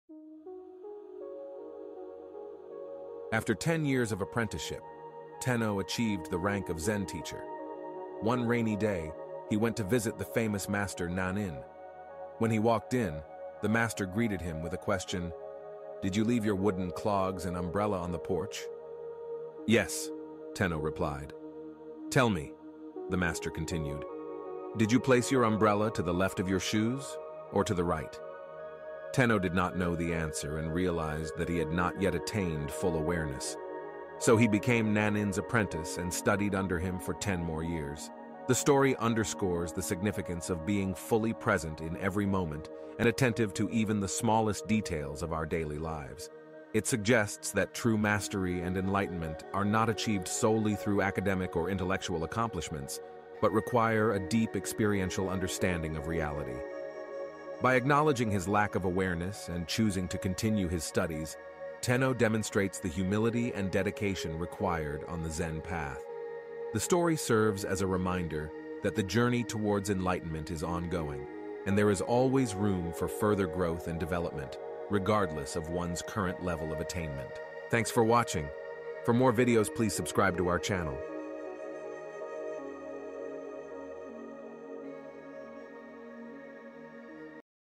Be Attentive: Steps to Enlightenment| 101 Zen Stories| Wisdom Tale
Tenno, after a decade of apprenticeship, achieved the rank of Zen teacher. One rainy day, he visited the renowned master Nan-in. Upon entering, Nan-in asked, "Did you leave your wooden clogs and umbrella on the porch?" Tenno replied affirmatively. The master then inquired about the placement of the umbrella—whether it was to the left or right of the shoes. Tenno was unsure, realizing he had not attained full awareness. He became Nan-in's apprentice for another ten years, recognizing the importance of being fully present and attentive to even the smallest details of daily life. The story teaches that true mastery and enlightenment require experiential understanding and continuous growth, transcending intellectual accomplishments.